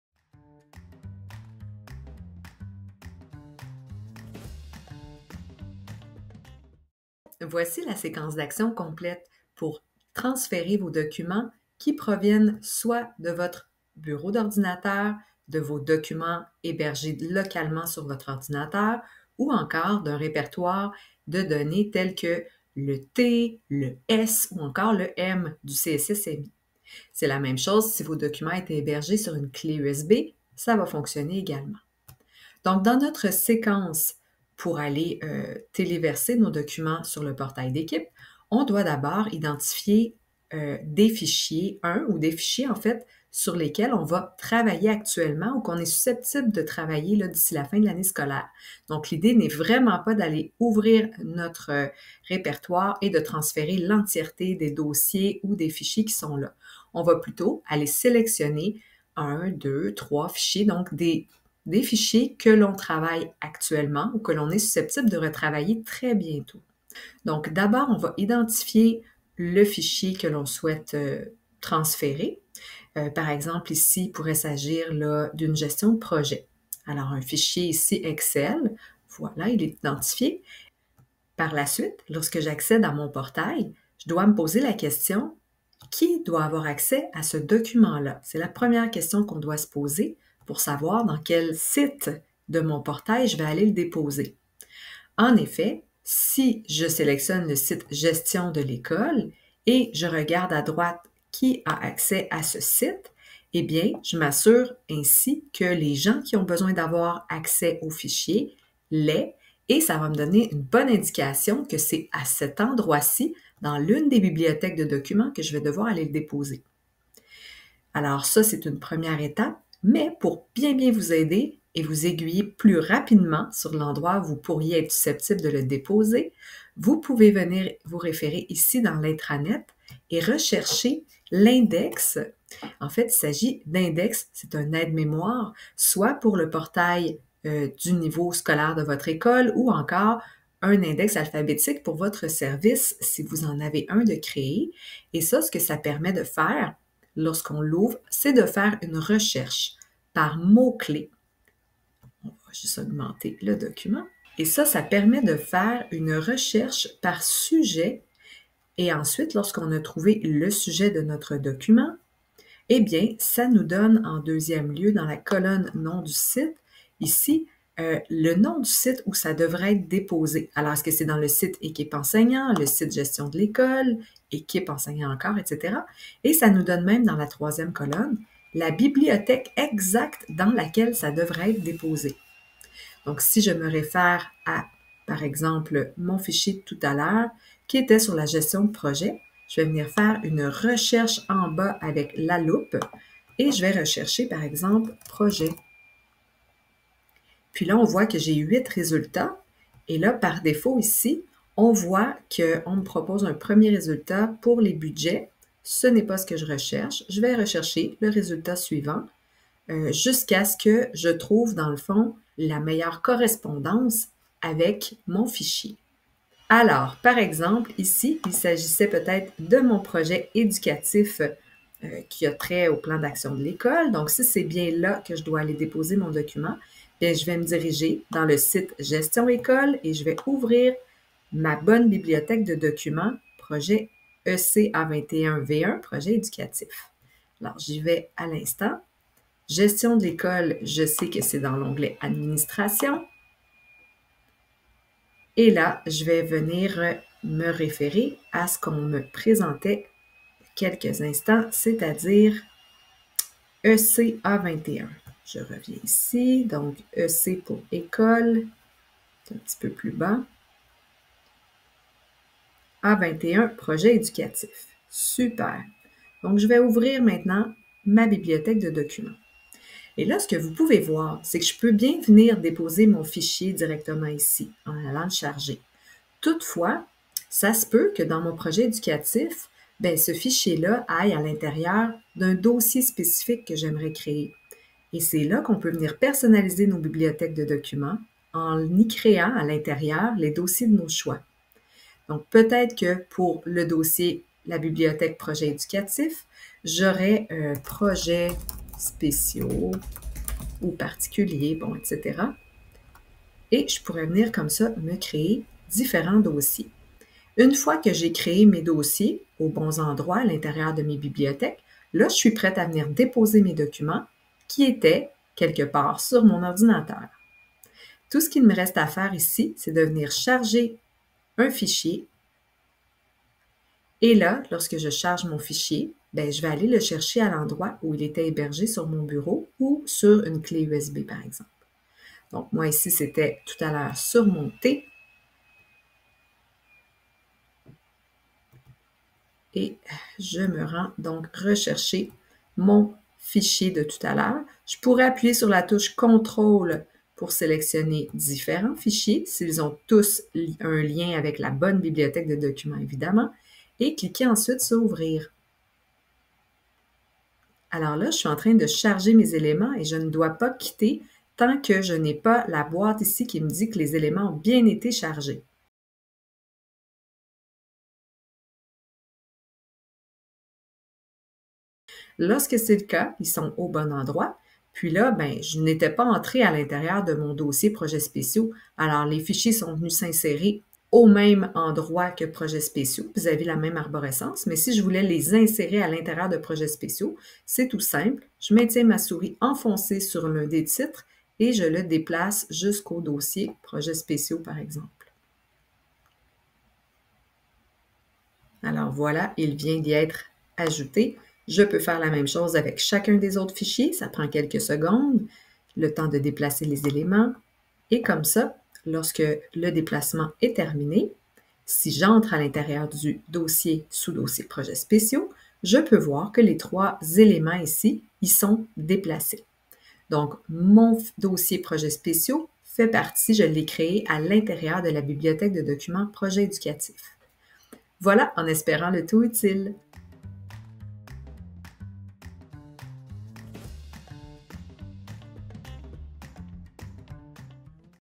Déplacer un fichier de l'ordinateur vers le portail d'équipe
Voici comment déplacer ou faire migrer vos documents présentement hébergés sur votre poste (bureau d'ordinateur, répertoire local comme le «S», le «T» ou le «M», ou encore une clé USB vers le portail d'équipe SharePoint.